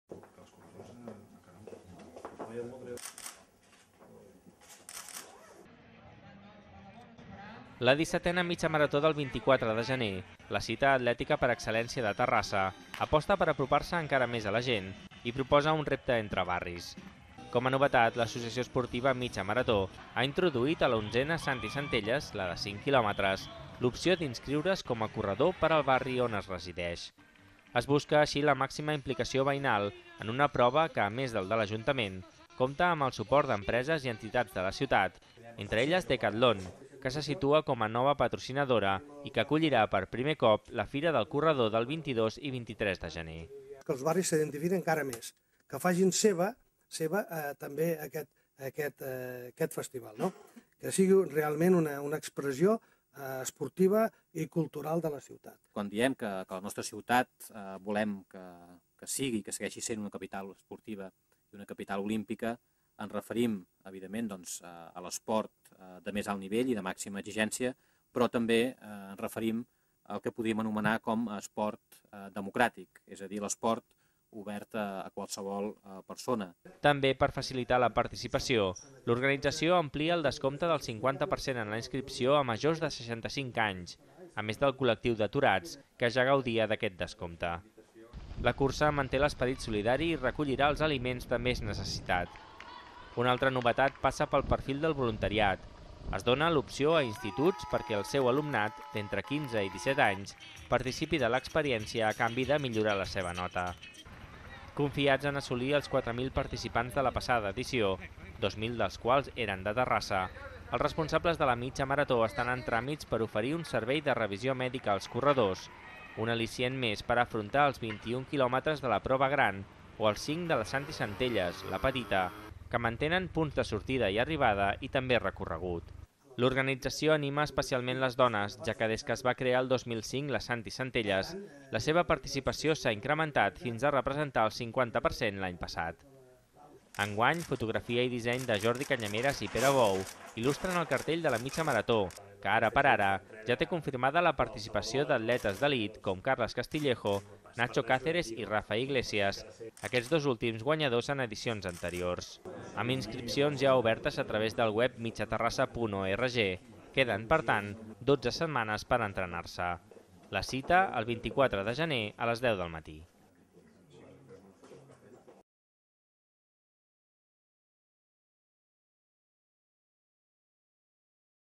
Presentació de la 17ª Mitja Marató Ciutat de Terrassa
La 17ª Mitja Marató i l’11ª Cursa Santi Centelles tindran lloc el proper 24 de gener, organitzades per l’Associació Esportiva Mitja Marató de Terrassa amb el suport de l’Ajuntament.